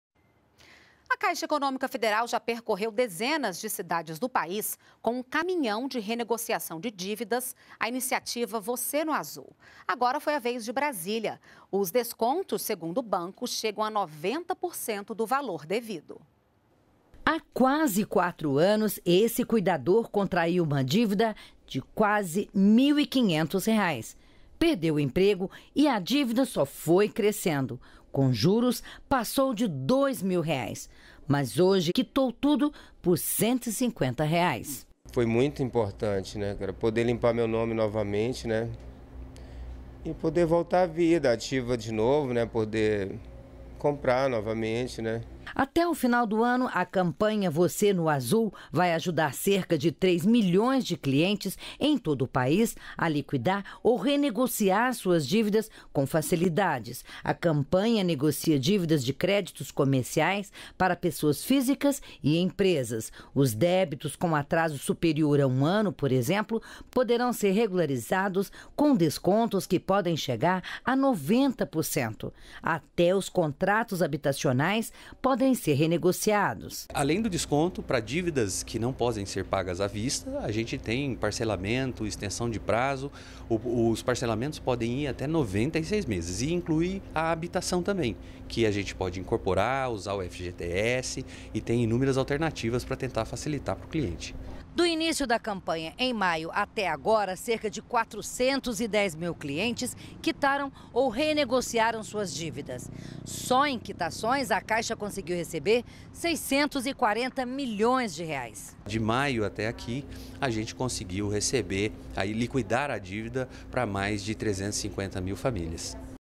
"Você no Azul" dá até 90% de desconto em dívidas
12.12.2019 - A Caixa Econômica Federal já percorreu dezenas de cidades do país com um caminhão de renegociação de dívidas, a iniciativa 'você no azul'. Agora foi a vez de Brasília. Os descontos, segundo o banco, chegam a 90% do valor devido.